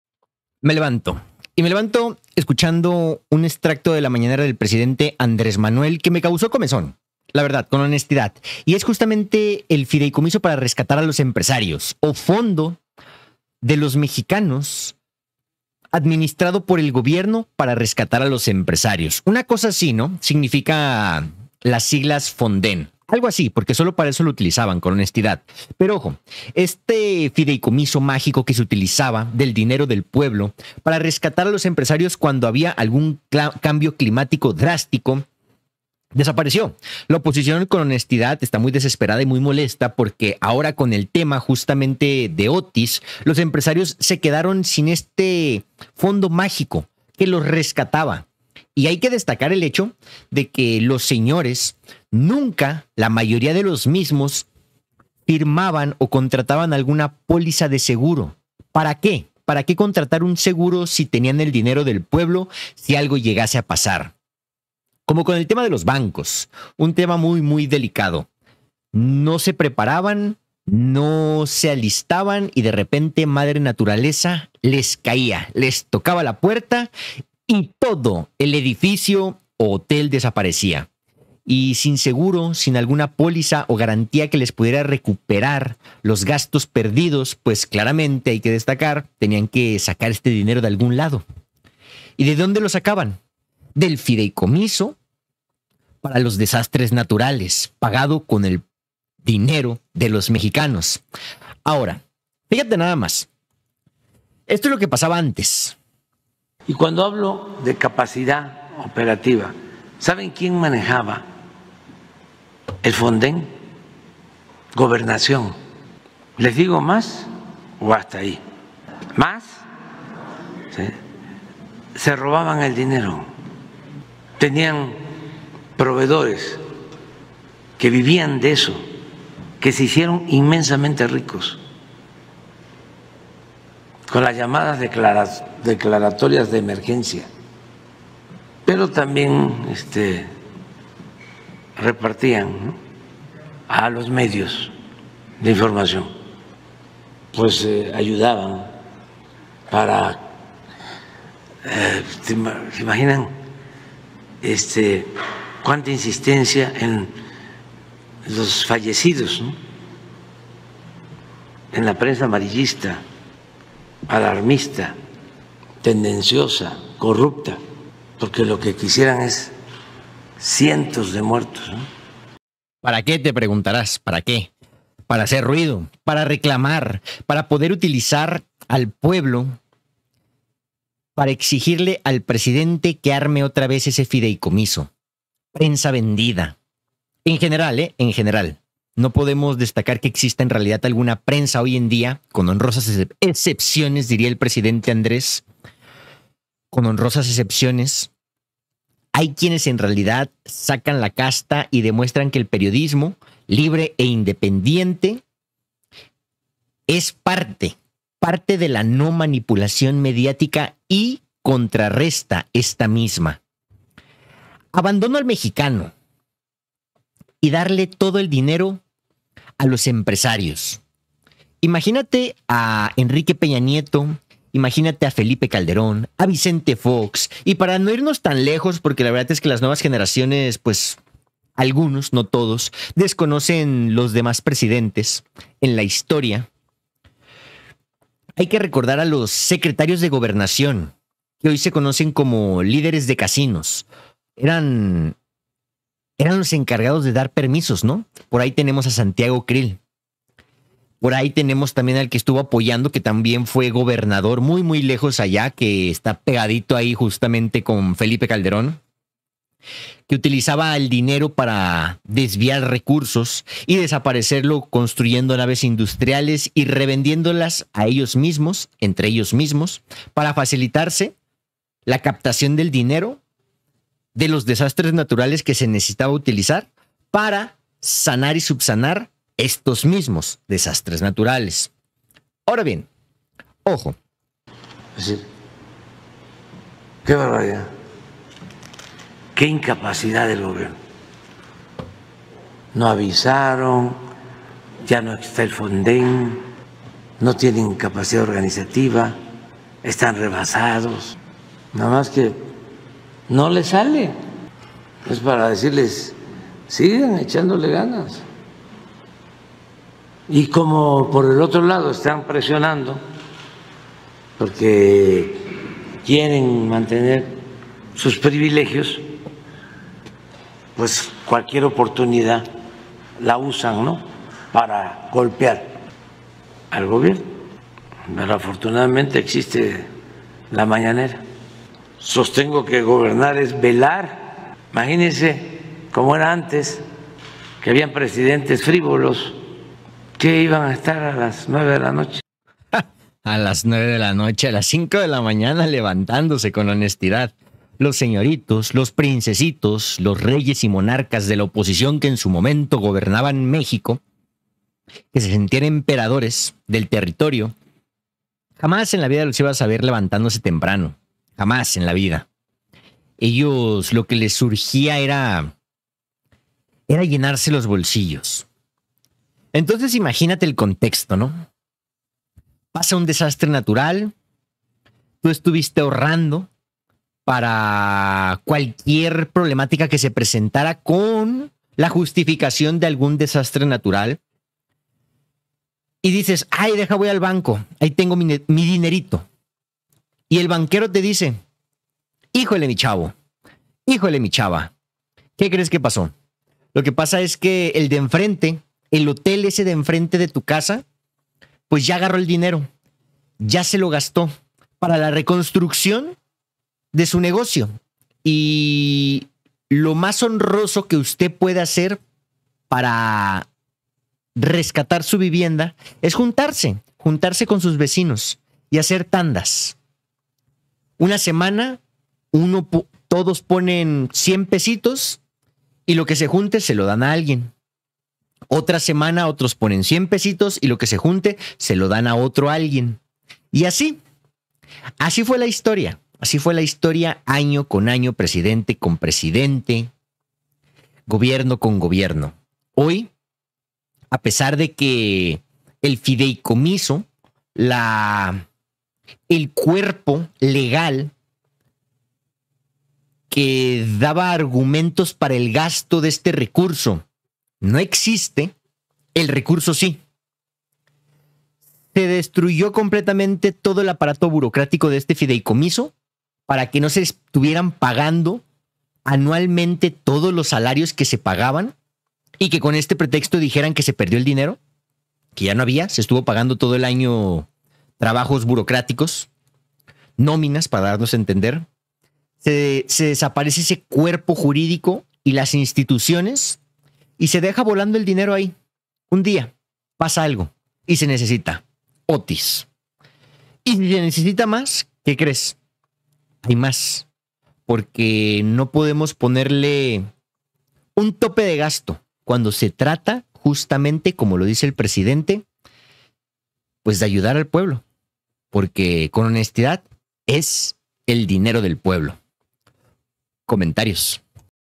Obrador le dice no a los empresarios
No habrá apoyo. ellos pueden pedir prestado o pagar de su bolsillo pero de el de el pueblo no..